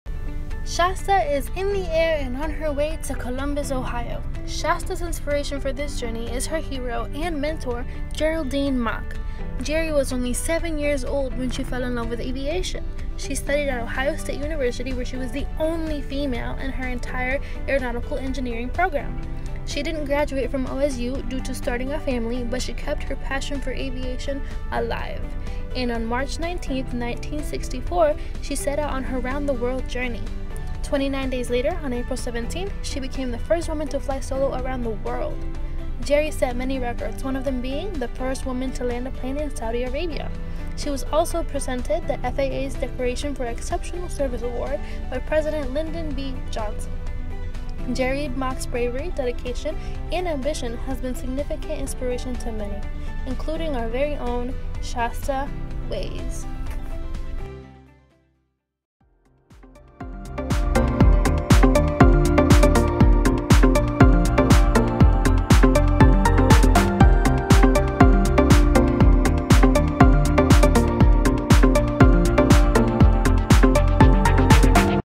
Celebrating Jerrie Mock: Columbus, Ohio (KCMH) Outreach
On May 13th, 2017, Shaesta Waiz departed for her flight around the world!

✈️🌎 First stop: Columbus, Ohio! 📍

Columbus is the home of the first woman to ever fly around the world solo, Geraldine "Jerrie" Mock! Learn about Jerrie's exploits right here!